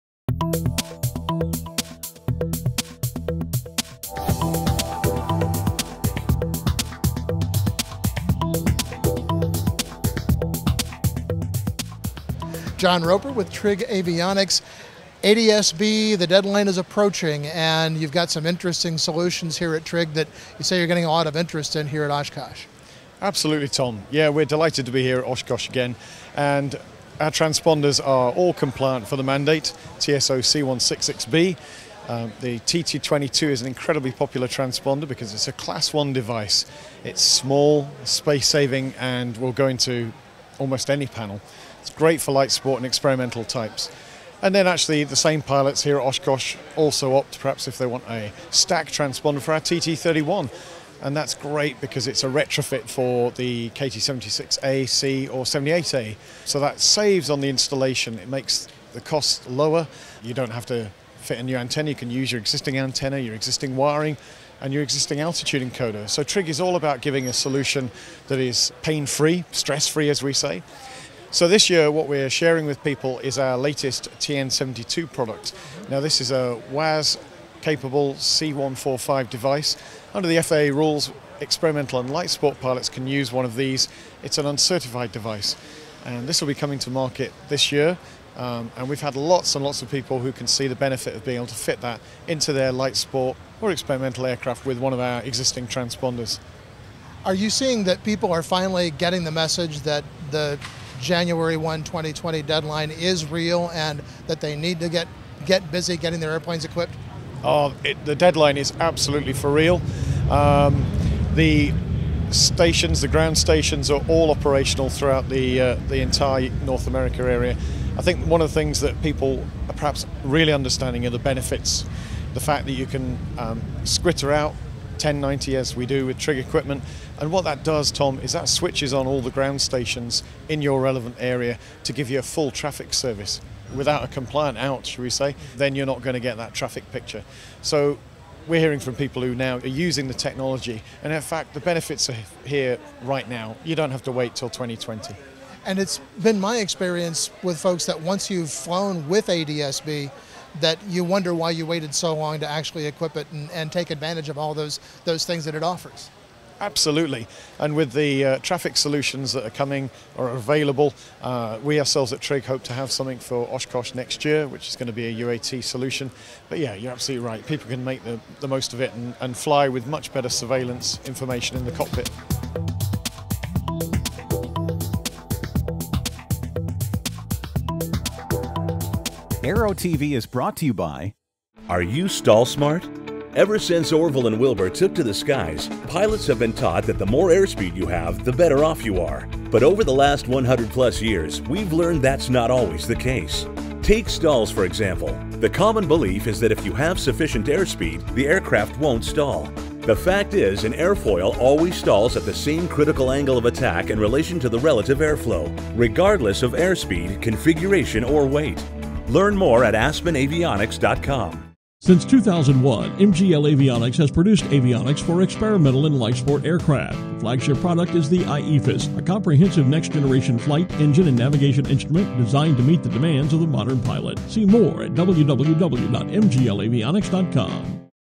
Aero-TV: The New TN72 & the TT22 - Trig Transponders Prep For ADS-B
Space Saving Transponders To Meet The 2020 Mandate…

While at the EAA AirVenture 2016, ANN News Editor, Tom Patton, checked in with Trig Avionics to see what’s new and to get the inside scoop on their products. Tom had the opportunity to speak with Trig Avionics Marketing Manager, John Roper, and in this discussion the topic is the interface of the transponder and ADS-B Out.

Roper starts the conversation by addressing experimental and light sport market and the application of their TT22 transponder. It is a TSO’ed, compact unit, that can fit into a standard instrument cut-out. The TT31 is a stacked configuration and is also a type certificated unit. And, if you want to keep your existing Trig transponder and simply upgraded it, the TN72 is a GPS WAAS add-on for Trig units.

As the conversation continues, Roper talks about the advantages of ADS-B out and how it can improve your flying experience. He offers some interesting insights about ADS-B, and sounds convincing when he says the 2020 mandate is real. This video provides a great view of the equipment and some good advice.